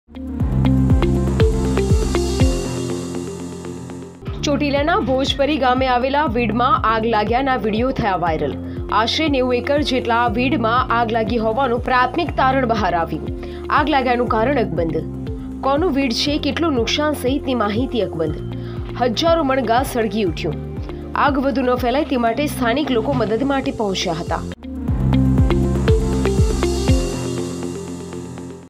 ચોટીલાના ભોજપરી ગામે આવેલ વિડમાં આગ લાગ્યાના વિડીયાઓ થયા વાયરલ
Trending Gujarat